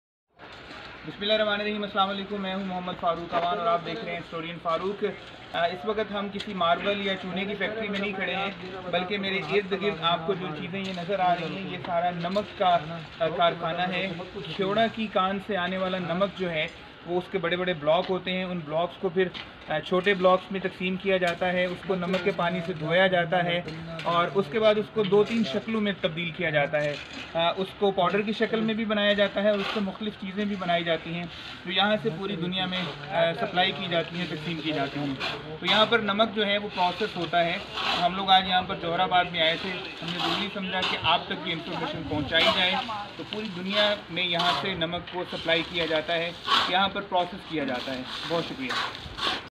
Salt Factory part one Qaida Abad Khoshaab#Shamas sb
factory is preparing and reshaping salt  as it got from mines
factory reshape export quality salt